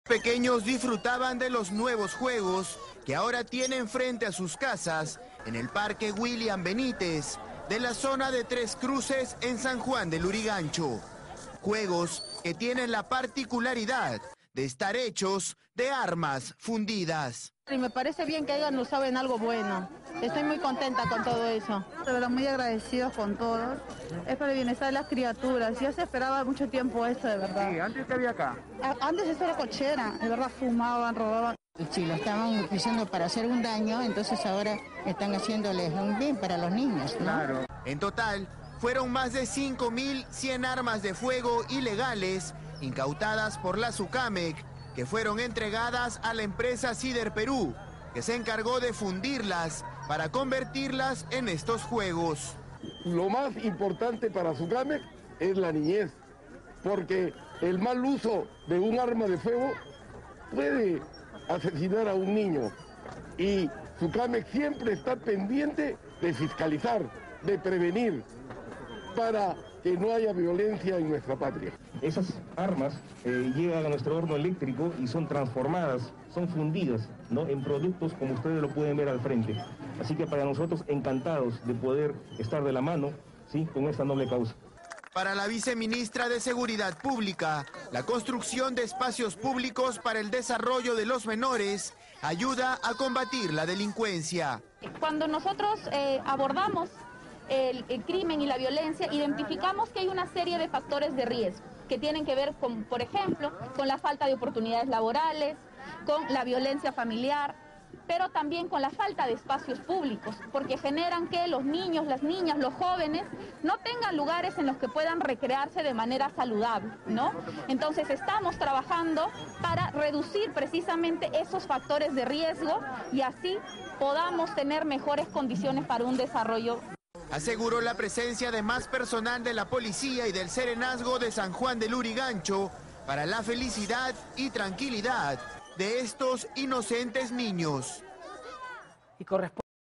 SJL: inauguran juegos recreativos y mini gimnasios con armas fundidas
Este jueves en San Juan de Lurigancho, la Superintendencia Nacional de Control de Servicios de Seguridad, Armas, Municiones y Explosivos de Uso Civil (Sucamec) y la empresa Siderperu donaron a más de 1,500 jóvenes y niños, mini gimnasios y juegos recreativos construidos con el acero fundido de armas de fuego decomisadas.

La donación se dio en cumplimiento del círculo virtuoso de la seguridad ciudadana en materia de armas de fuego, el cual consiste en retirar de la sociedad las armas que significan un riesgo, para fundirlas y convertirlas en implementos deportivos y recreativos que retornan a la sociedad en beneficio de los jóvenes y niños en situación de vulnerabilidad.

Para la viceministra de Seguridad Pública, Gabriela Paliza, la construcción de espacios públicos para el desarrollo de los menores ayuda a combatir la delincuencia.

Asimismo, Paliza aseguró la presencia de más personal de la policía y del serenazgo de SJL para la felicidad y tranquilidad de los más pequeños.